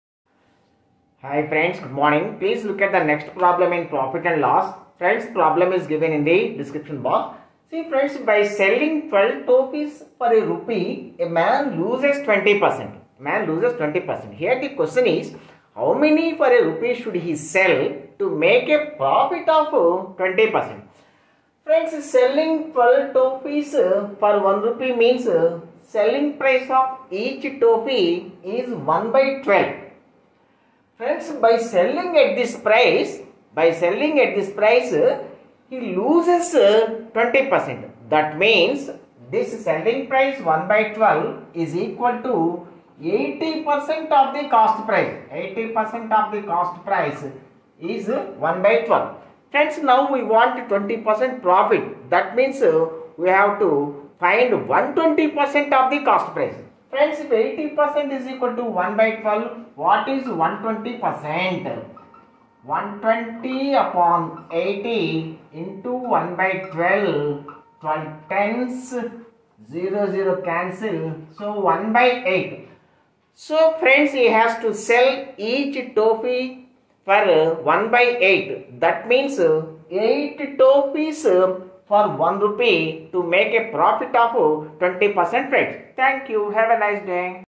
By selling 12 toffees for a rupee, a man loses 20%. How many toffees for a rupee should
By selling 12 toffees for a rupee, a man loses 20%. How many toffees for a rupee should he sell to get a gain of 20%?